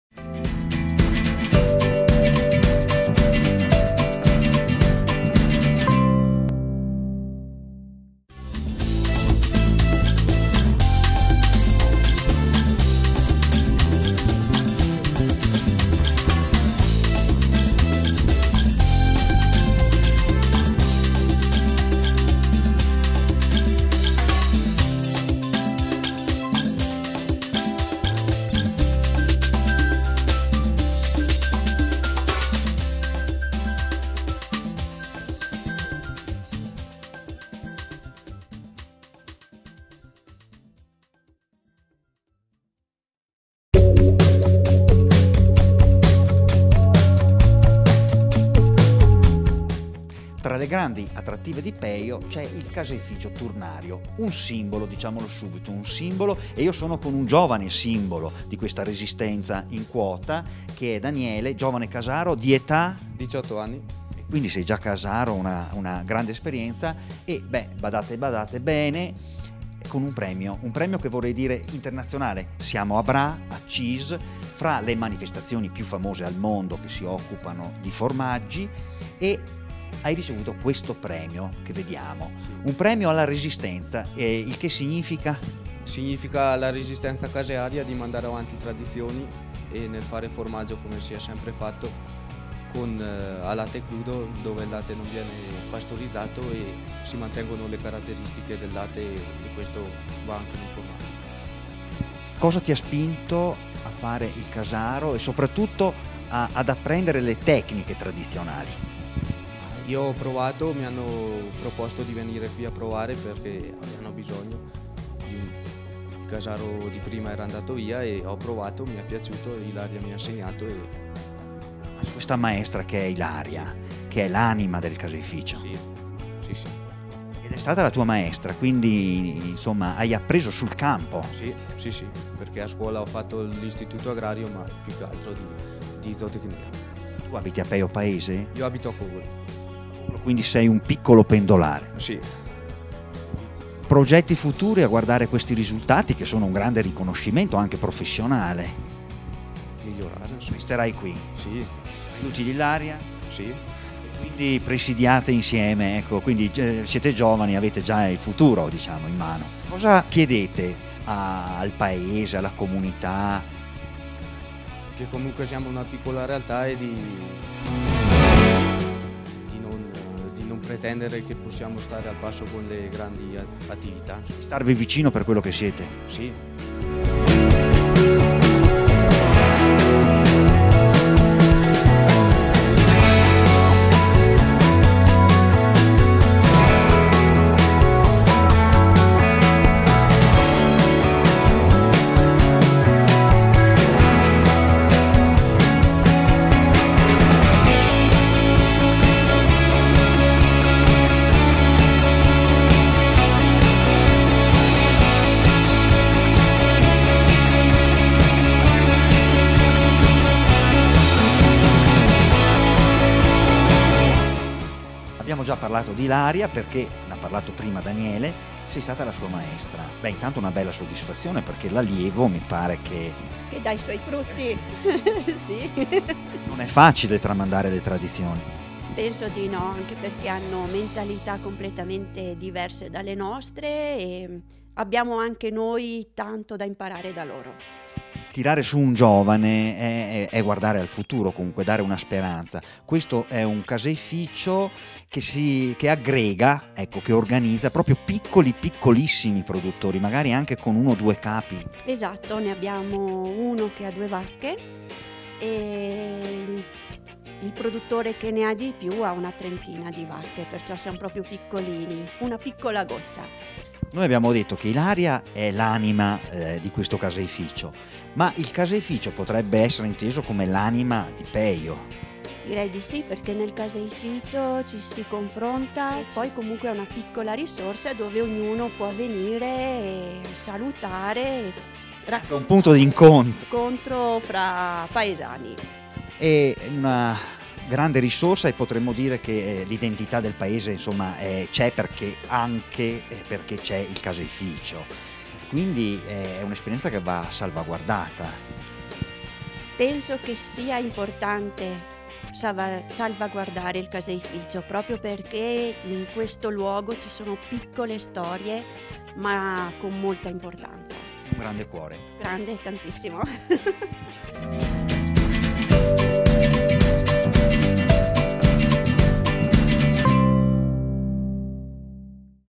Caseificio Turnario di Pejo
Il Caseificio Turnario di Pejo è l’ultimo caseificio turnario rimasto in Trentino.
Visitare questo caseificio è un’immersione nella storia del formaggio trentino. Qui infatti sia il modello amministrativo che quello produttivo si rifanno a tradizione quasi secolari.
Il formaggio viene prodotto ogni giorno dal giovanissimo casaro con il latte di vacca che arriva dai cinque soci conferitori. Ciascuna “caserada” viene lavorata solo con latte crudo (non pastorizzato), che mantiene pertanto tutte le proprie caratteristiche organolettiche. La caserada, a turno, appartiene ai soci che ricevono formaggio in quantità proporzionale al latte conferito. Una parte di formaggio viene poi trattenuta per essere venduta nel punto vendita.
Tra i prodotti principali del Caseificio, citiamo sicuramente il Casolet di Malga a latte crudo, Presidio Slow Food.